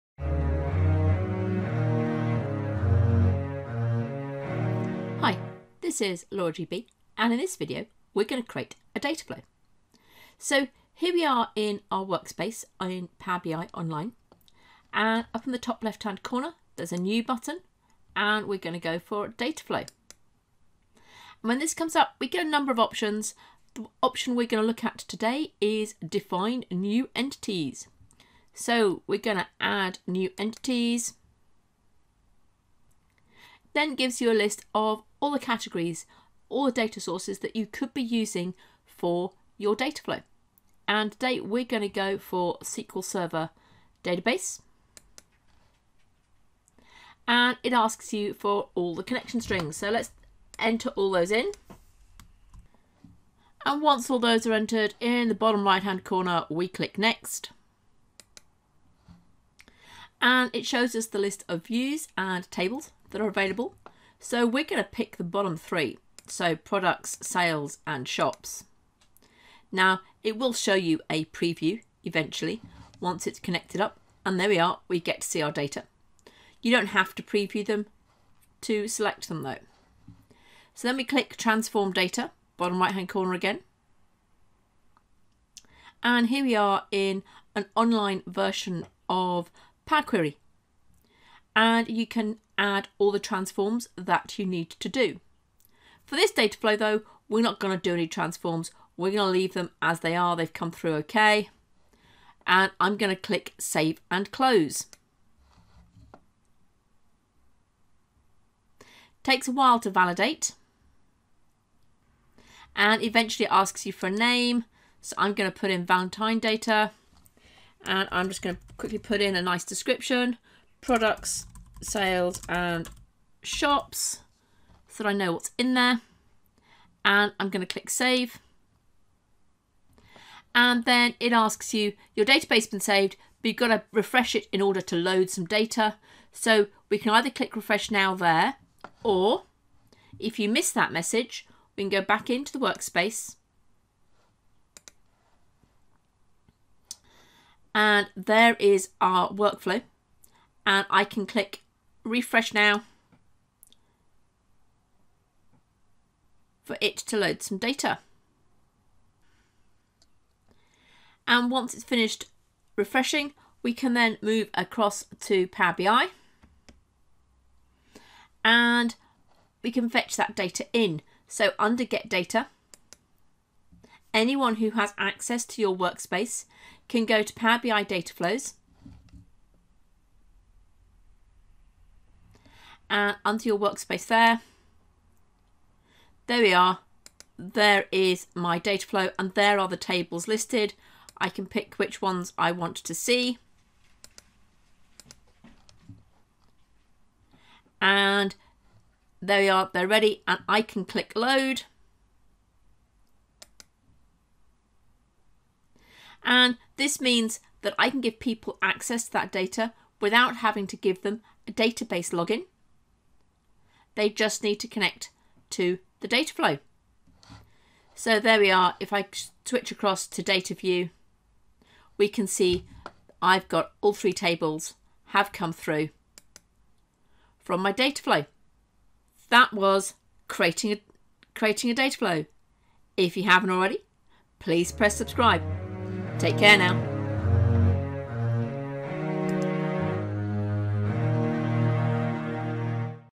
Power BI - Create a Dataflow
Creating a dataflow that connects to SQL Server data and then connecting the dataflow to a Power BI report.